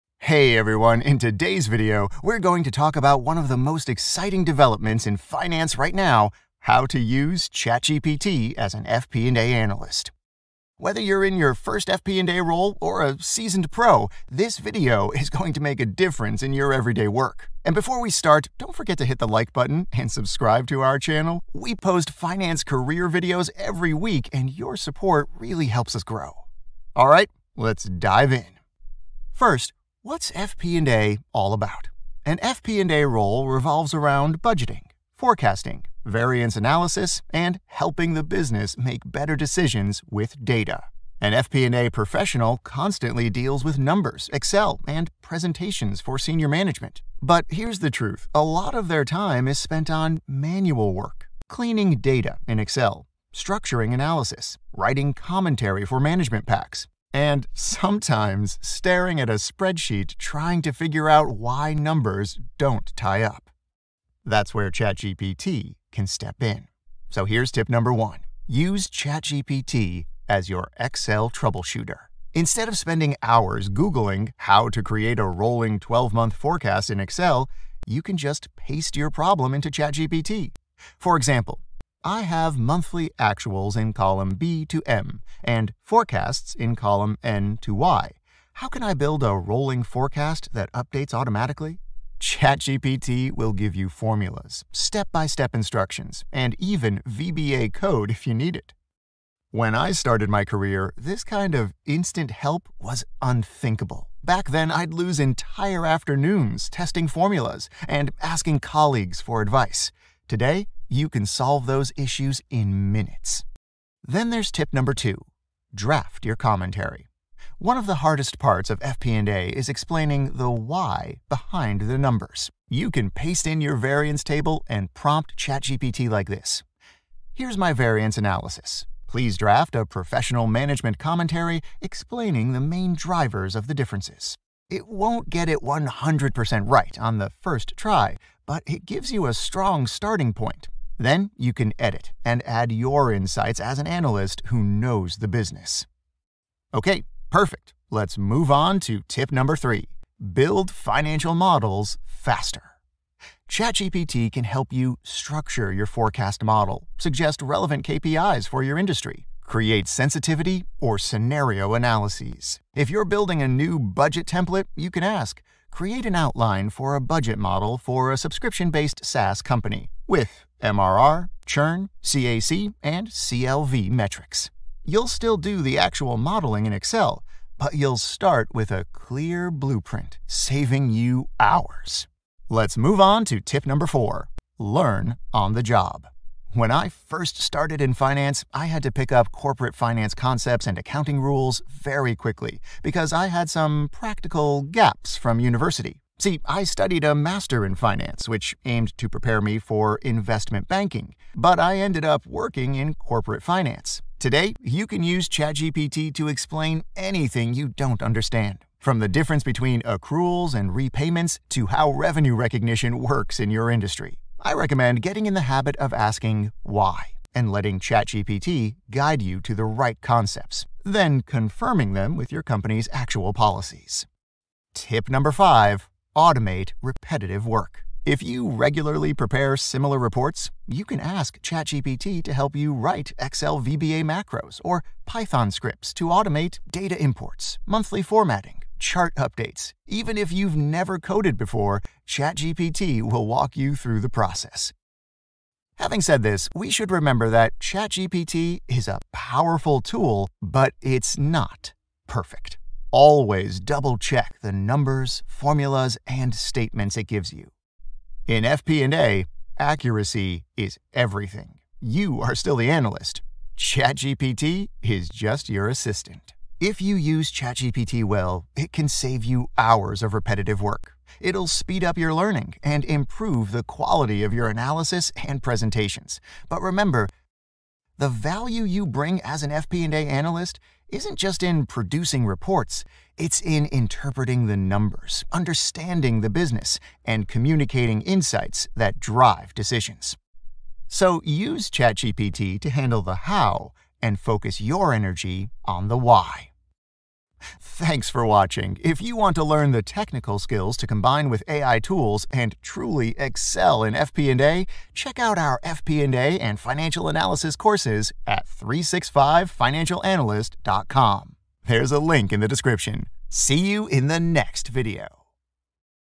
How to Use ChatGPT as an FP&A Analyst (2025)
🥳 Access all 365 Financial Analyst courses 100% for free — November 6–21!

This video shows how ChatGPT can make FP&A work faster and easier. FP&A analysts spend much of their time on budgeting, forecasting, variance analysis, and reporting—but also on manual tasks like fixing Excel formulas, writing commentary, and formatting reports. ChatGPT helps by acting as an Excel troubleshooter, drafting variance commentary, and speeding up financial model design. It can also explain complex finance concepts on demand and even generate VBA or Python scripts to automate repetitive reporting tasks.
While accuracy must always be checked, ChatGPT saves hours and reduces busy work. That frees analysts to focus on the real value of FP&A: interpreting results, understanding the business, and driving better decisions.

Watch to the end, and you’ll walk away with ready-to-use tips and tricks to make your daily tasks more efficient.